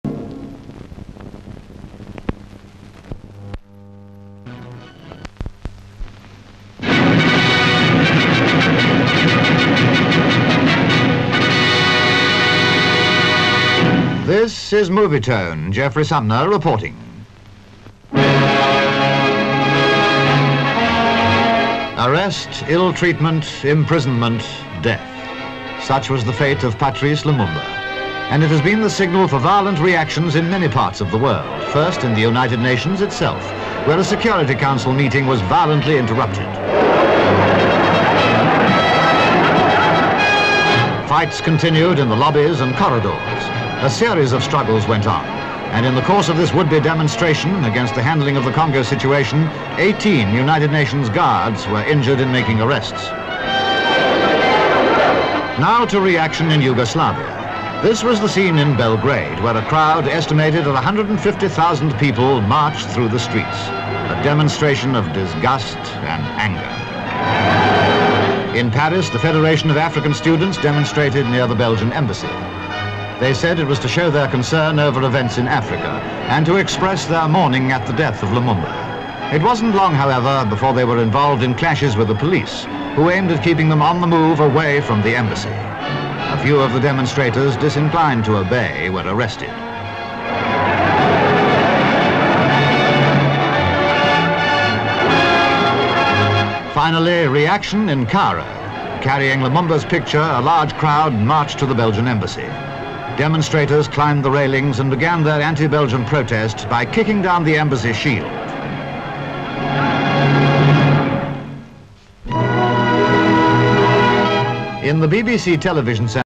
LUMUMBA - VIOLENT REACTION
(20 Feb 1961) Arrest, ill-treatment, imprisonment, death: such was the fate of Patrice Lumumba. It has been the signal for violent reactions in many parts of the world. First, in the United Nations itself, where a Security Council meeting was violently interrupted. 18 UN guards were injured in making arrests. Now, to reaction in Yugoslavia. This was the scene in Belgrade, where a crowd estimated at 150,000 marched through the streets. In Paris, the Federation of African students demonstrated near the Belgian Embassy. Finally, reaction in Cairo. Carrying Lumumba's picture, a large crowd marched to the Belgian Embassy, and demonstrators climbed the railings and began their anti-Belgian protest by kicked down the Embassy shield.